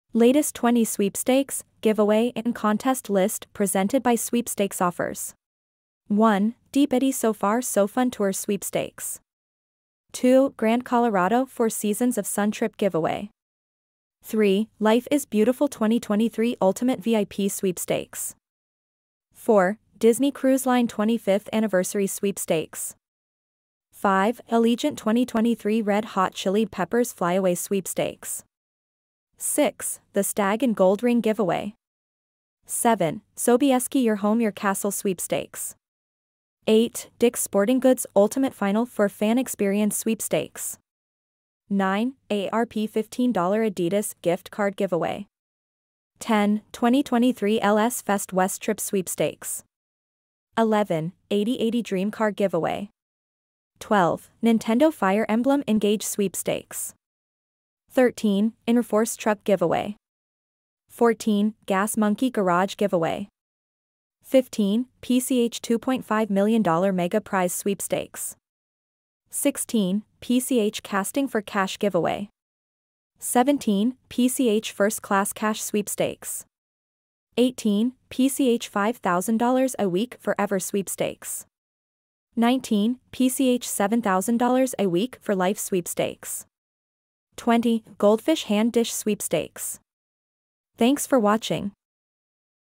Latest 20 Sweepstakes, Giveaway and Contest List presented by sweepstakesoffers | 27 February 2023
1. Deep Eddy SoFar SoFun Tour Sweepstakes

3. Life is Beautiful 2023 Ultimate VIP Sweepstakes

4. Disney Cruise Line 25th Anniversary Sweepstakes

5. Allegiant 2023 Red Hot Chili Peppers Flyaway Sweepstakes

7. Sobieski Your Home Your Castle Sweepstakes

8. DICK'S Sporting Goods Ultimate Final Four Fan Experience Sweepstakes

10. 2023 LS Fest West Trip Sweepstakes

12. Nintendo Fire Emblem Engage Sweepstakes

15. PCH $2.5 Million Mega Prize Sweepstakes

17. PCH First Class Cash Sweepstakes

18. PCH $5000 A Week Forever Sweepstakes

19. PCH $7000 A Week For Life Sweepstakes

20. Goldfish Hand Dish Sweepstakes